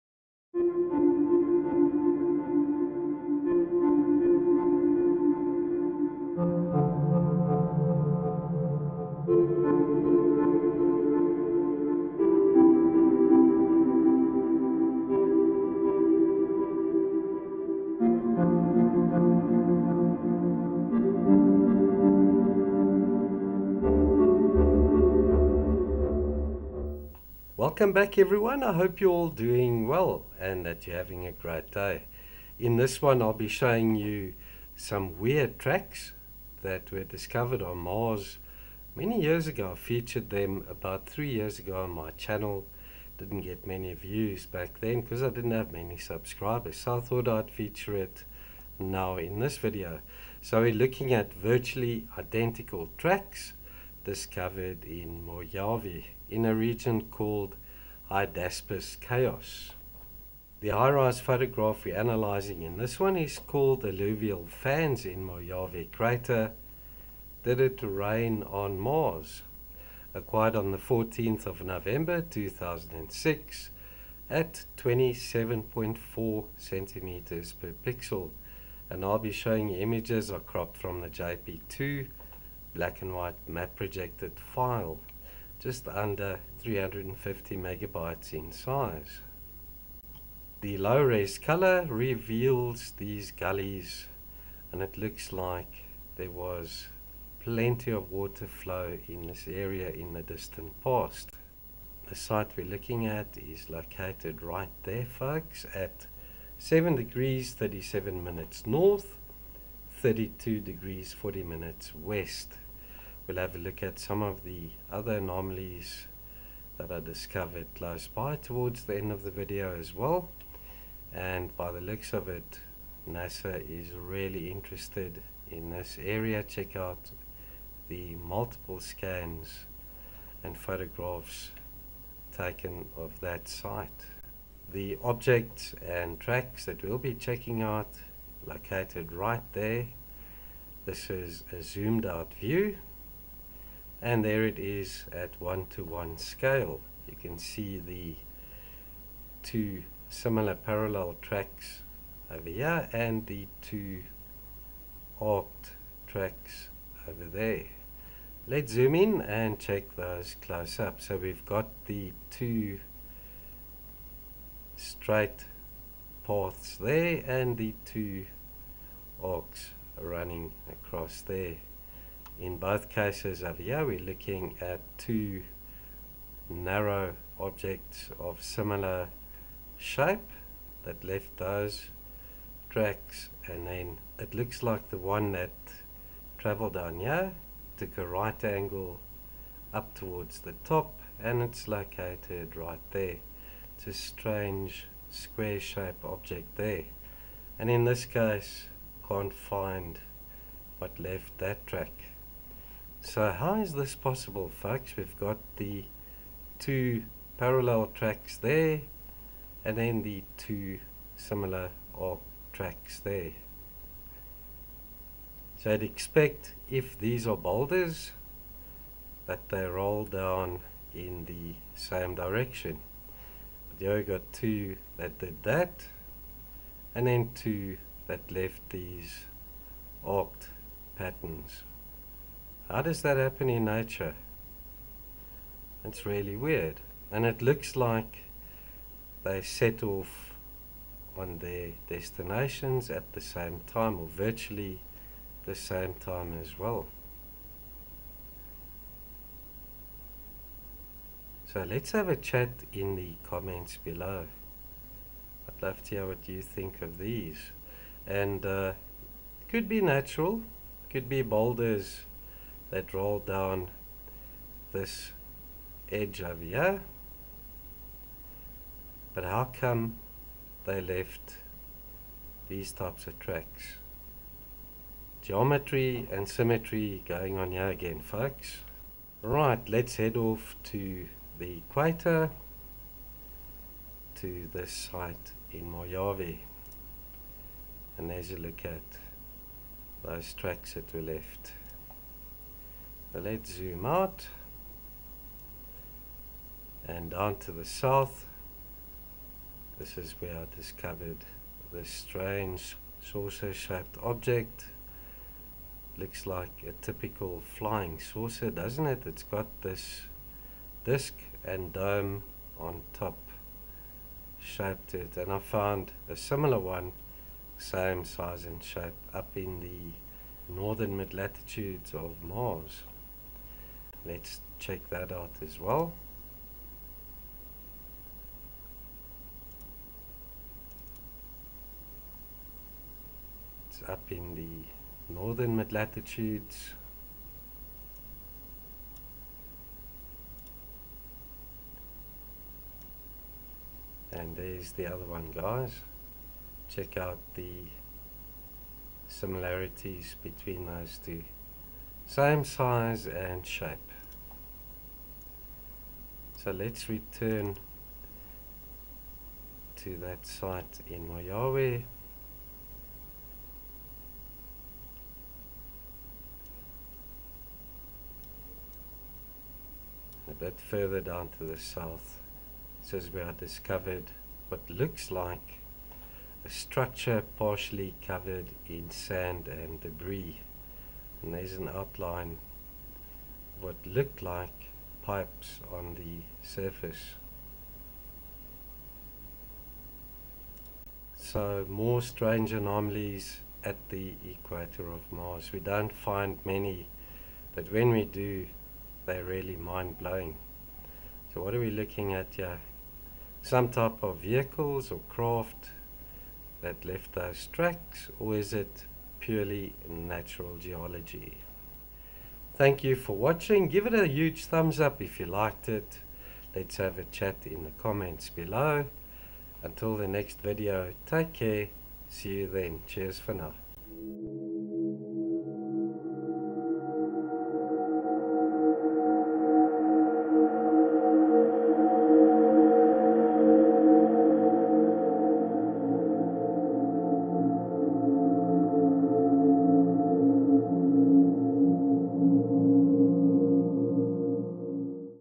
Virtually identical Tracks Discovered in Mojave on Mars
The photograph analyzed here (PSP_001415_1875) was acquired by NASA’s Mars Reconnaissance Orbiter on 14 November 2006 and is described as ‘Alluvial Fans in Mojave Crater: Did It Rain on Mars?.’

Here I feature an anomaly I discovered at a new site in in Mojave in the region Hydaspis Chaos. Two sets of two tracks can be seen, one set of tracks are parallel and the other set of tracks follows the same arc. If these are boulders that rolled down the slope why don't they all four follows the same trajectory, after all they have the same origin points? There is symmetry and geometry at play here again.

My Blog Post featuring: 'Virtually identical Tracks Discovered in Mojave on Mars:'

My previous Blog and video featuring this anomaly and others in the same photograph I didn't cover here: